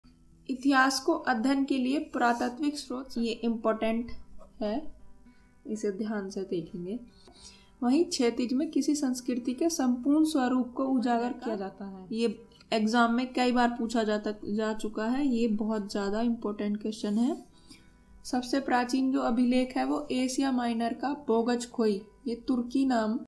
प्राचीन भारतीय इतिहास के पुरातात्विक स्त्रोत
prachin bharatiya itihas ke puratatvik strot
पुरातात्विक स्रोत
पुरातात्विक स्रोत के रूप में अभिलेख
बोगजकोई अभिलेख
अशोक के अभिलेख
रुद्रदामन का जूनागढ़ अभिलेख
गरुड़ स्तंभ

Hi everyone,

This channel is for the history.

अगर आप इसी प्रकार की Video चाहते हो तो हमारे